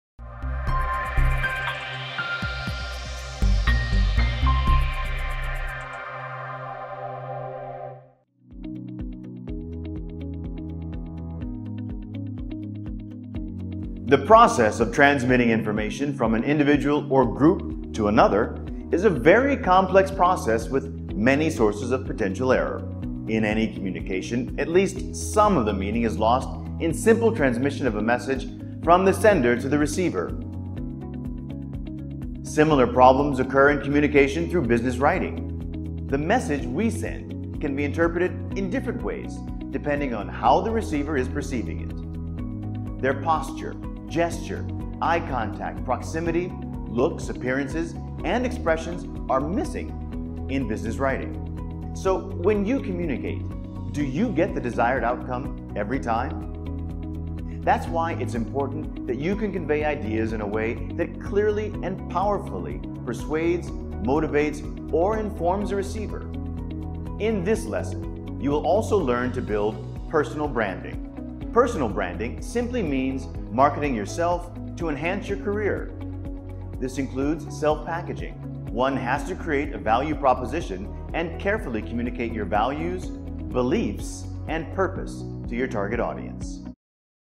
Business Power Skills: Business Skills - Training Course
This course is available from MasteryTCN through its network of Channel Partners

Being able to write succinctly and objectively are powerful assets no matter where you may find yourself in your career. This course discusses business writing and personal branding, and the interplay of these important components for developing professionalism and complementing one's skill set. Utilize this course at any level within your organization to help employees improve their business writing and hone their personal brand.